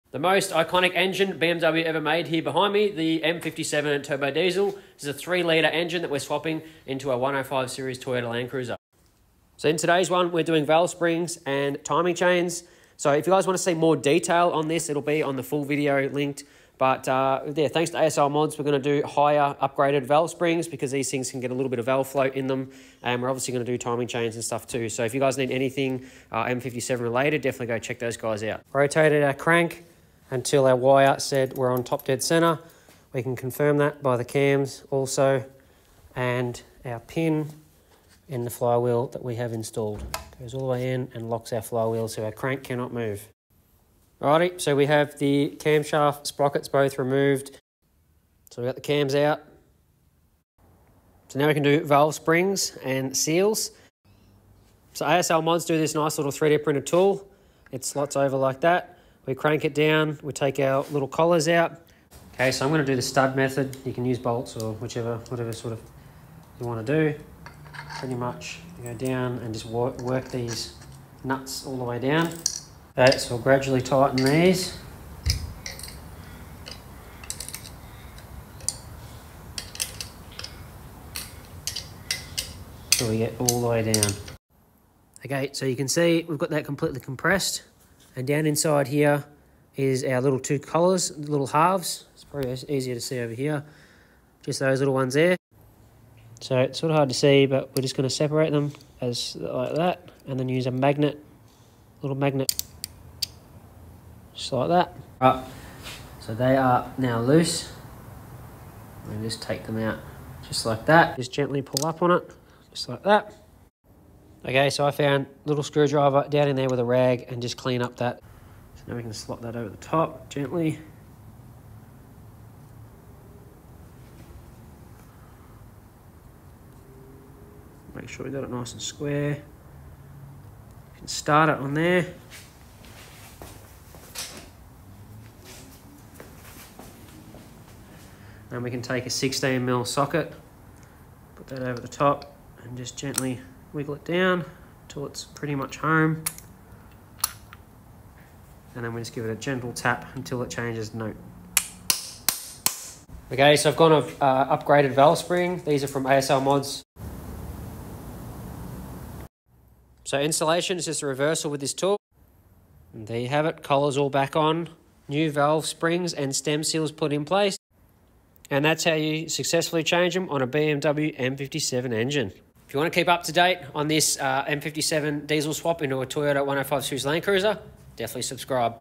M57 Diesel Swapping Landcruiser - Valve UPGRADE
UPGRADING Valve springs and replacing valve stem seals on a M57 engine. We are swapping this diesel into a landcruiser. Full video linked.